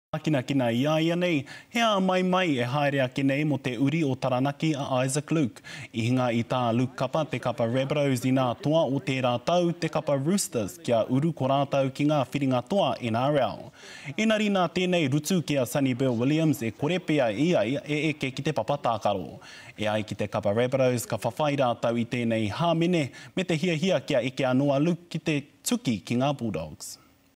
Issac Luke’s grand final fate to be decided
Taranaki descendant Issac Luke faces a nervous 24 hours. Luke's Rabbitohs team easily beat last year's premiers the Roosters to earn a spot in the NRL grand final, but he’s been put on report over a tipping tackle against Sonny Bill Williams that may get the hooker excluded from the big game.